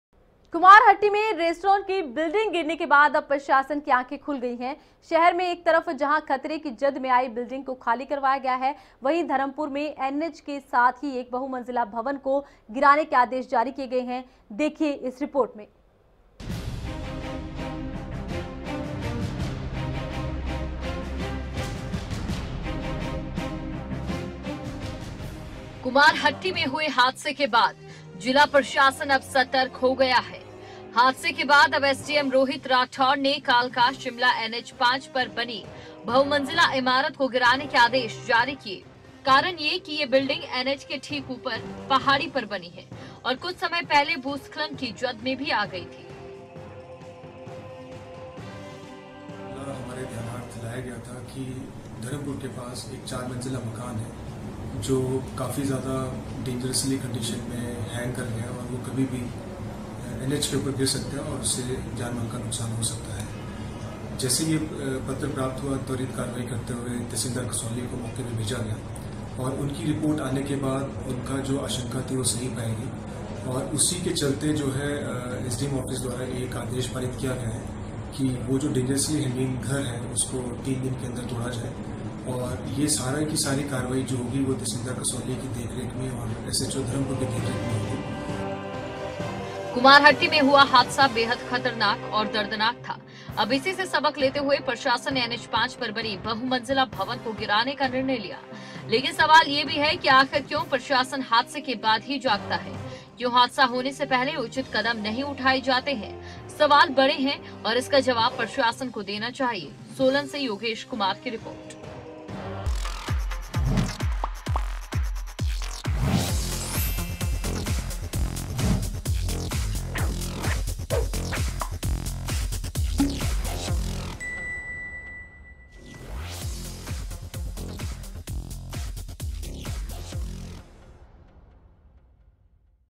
कुमारहट्टी हादसे के बाद प्रशासन शख्त, डेंजर जोन में आने वाले इमारत को गिराने के आदेश
कुमारहट्टी में रेस्टोरेंट की बिल्डिंग गिरने के बाद अब प्रशासन की आंख खुली है. शहर में एक तरफ जहां खतरे की जद में आई बिल्डिंग को खाली करवाया गया है, वहीं धर्मपुर में एनएच के साथ एक बहुमंजिला भवन को गिराने के आदेश जारी किए गए हैं